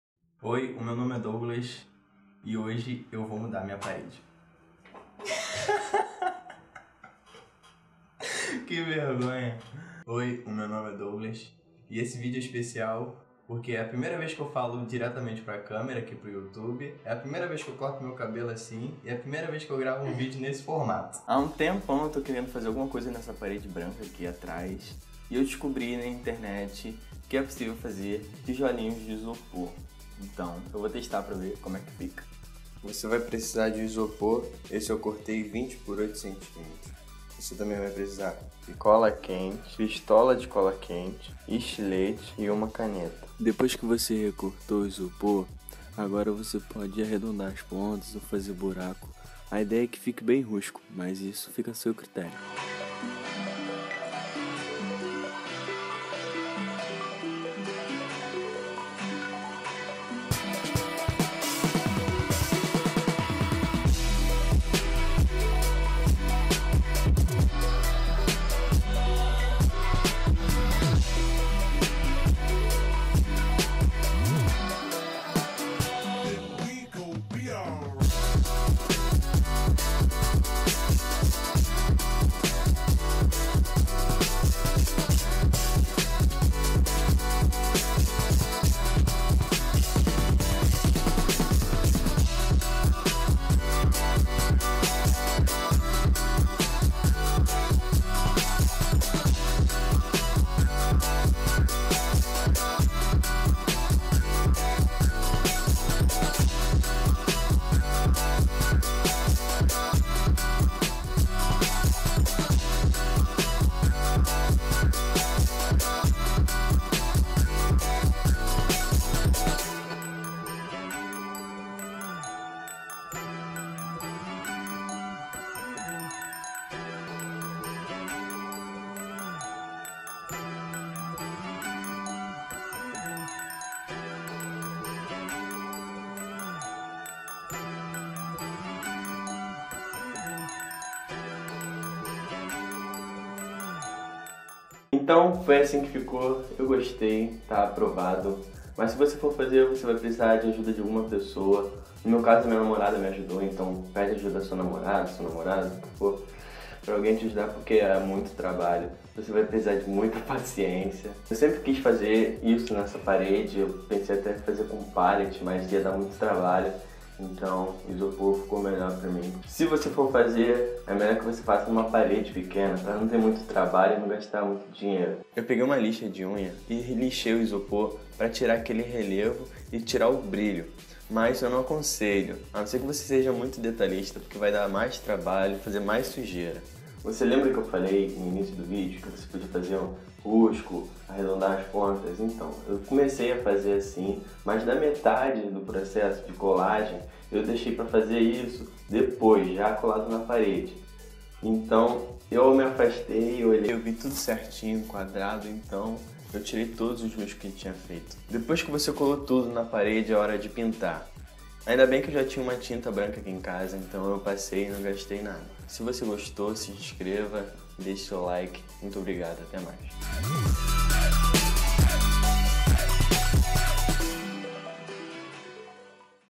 PAREDE DE TIJOLINHOS COM ISOPOR
No meu primeiro vlog (tenha paciência), mostro uma maneira fácil
e barata de fazer aquela parede de tijolinhos. Além de ficar bastante real, ajuda no isolamento acústico e térmico.
Qualquer dúvida pode perguntar nos comentários.